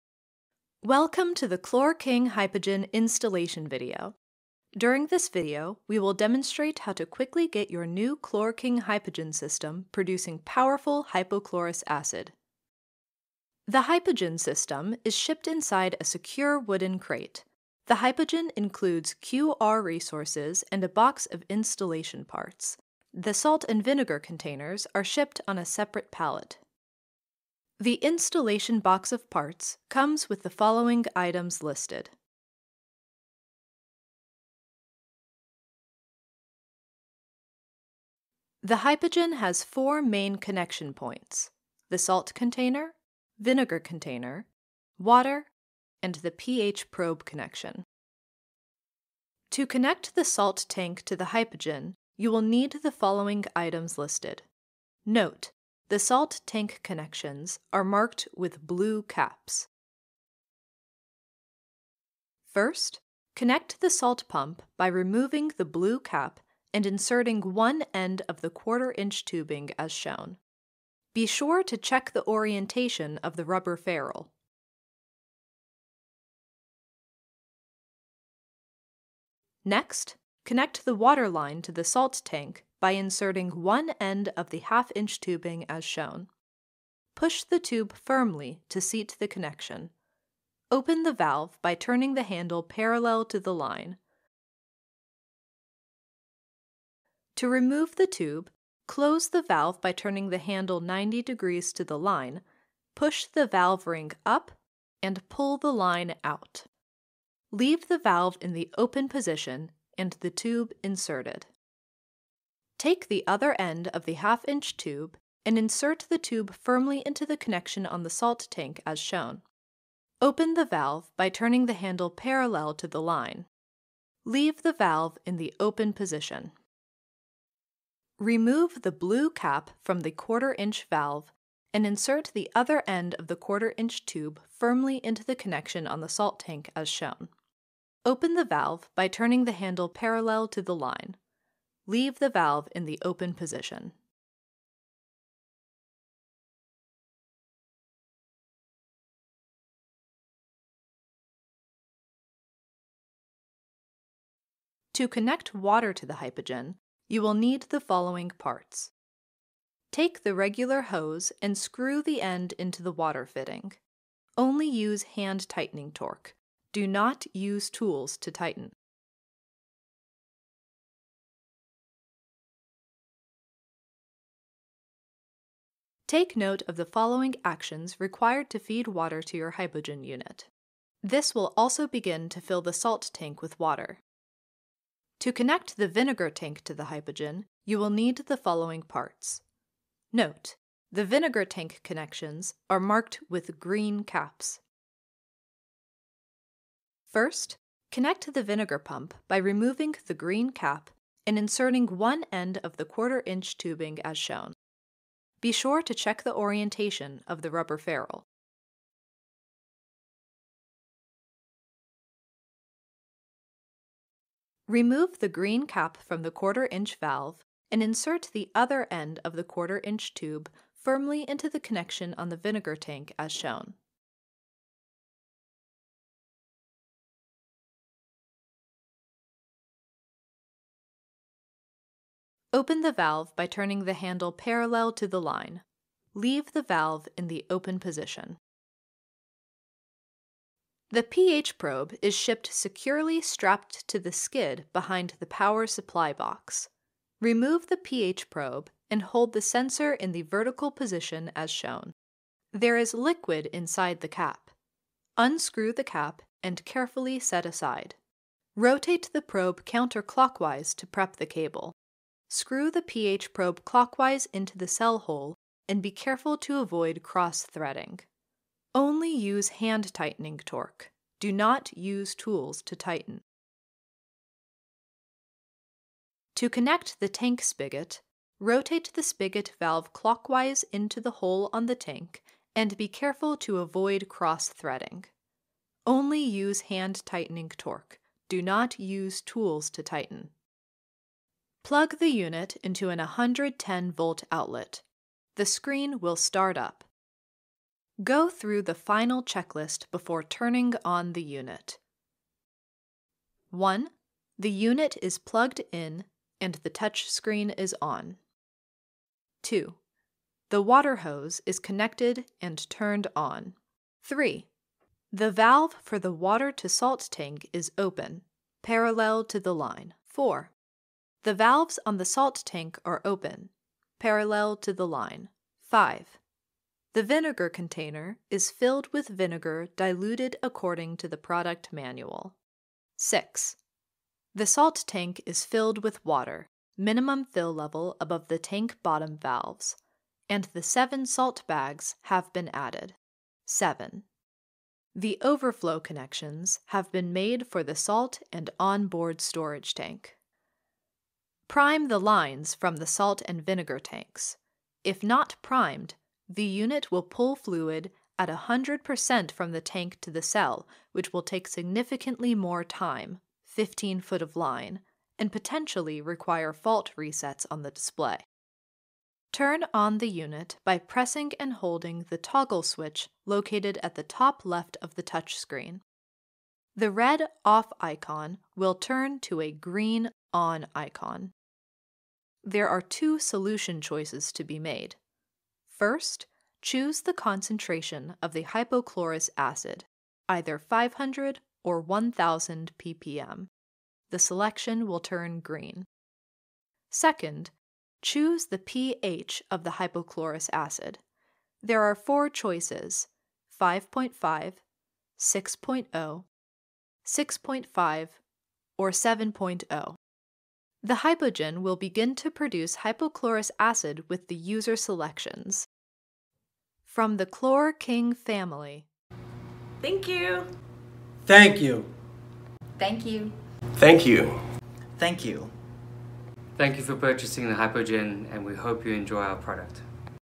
ChlorKing HYPOGEN Installation Video Auto
The ChlorKing HYPOGEN installation video aids customers in setting up their HYPOGEN systems to produce powerful hypochlorous acid.